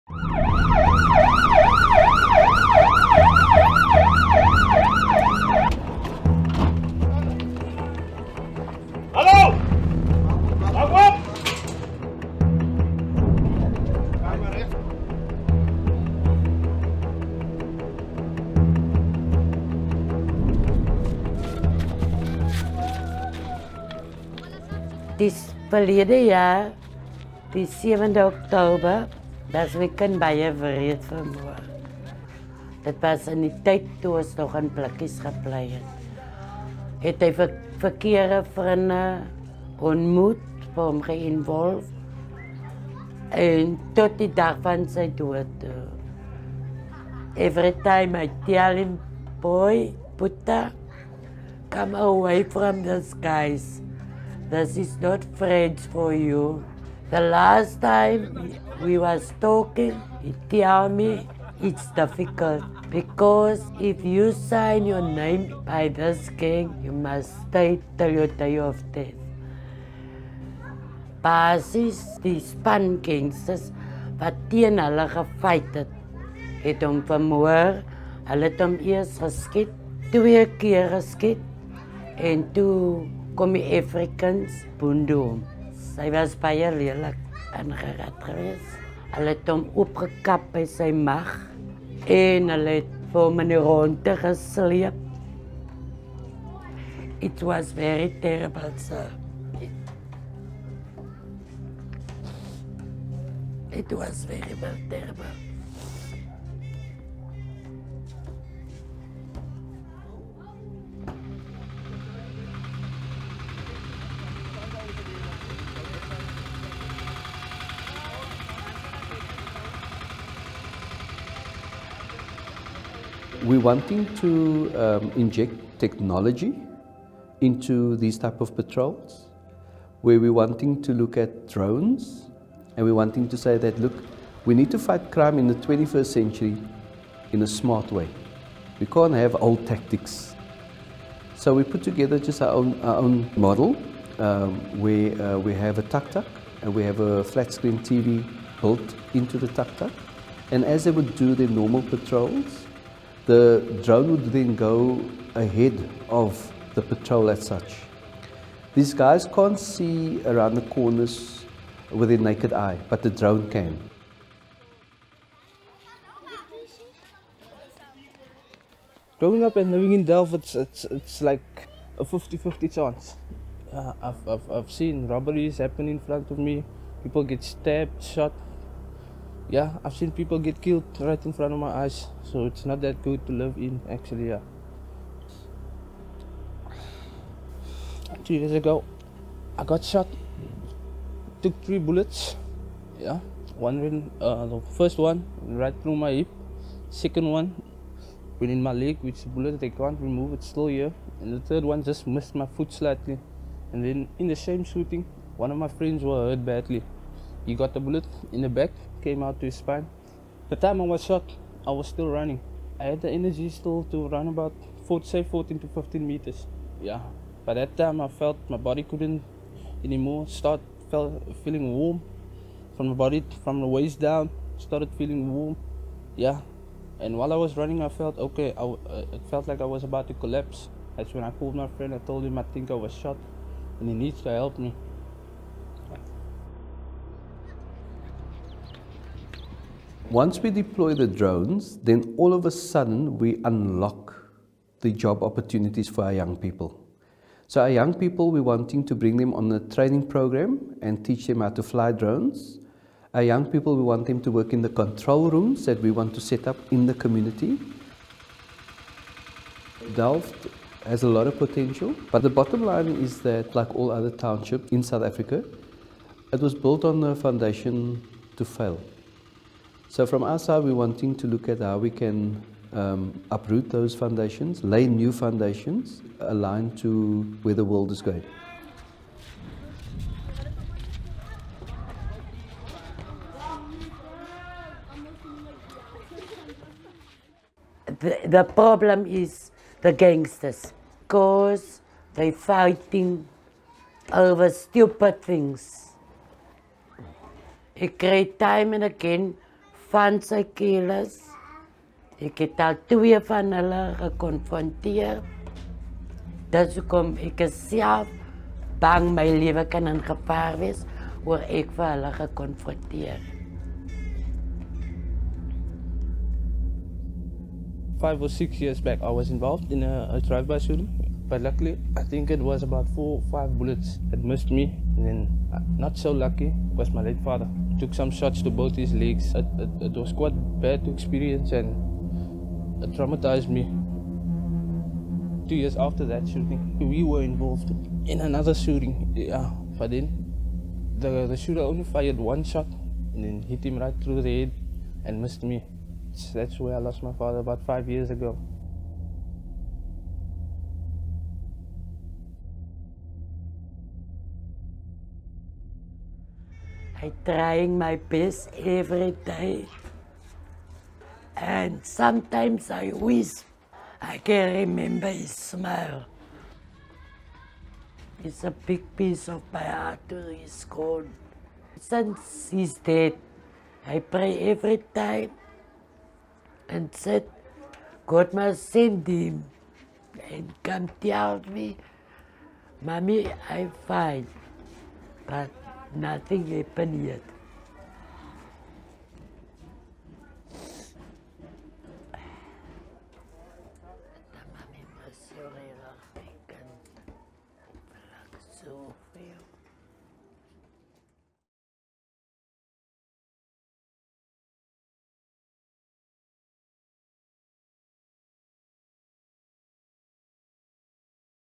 Drones in Delft: Using tech to fight Cape Town gangsterism
The community of Delft on the Cape Flats is ranked fifth highest in SA for its murder rate, which has become a grim daily reality for its residents. Now, the community policing forum is turning to technology to try to turn the tide against the scourge of gangsterism.